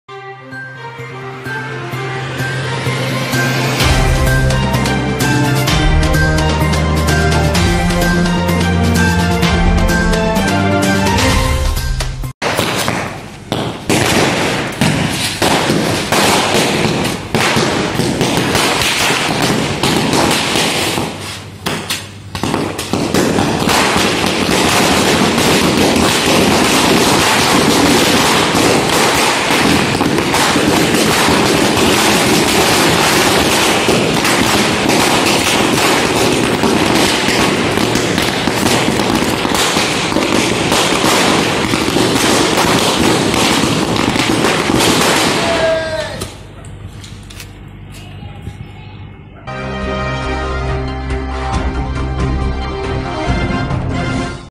India vs Pakistan World Cup | भारताच्या विजयानंतर वसईत फटाके फोड़ून केला आनंद व्यक्त
भारताच्या विजयानंतर वसईत फटाके फोड़ून केला आनंद व्यक्त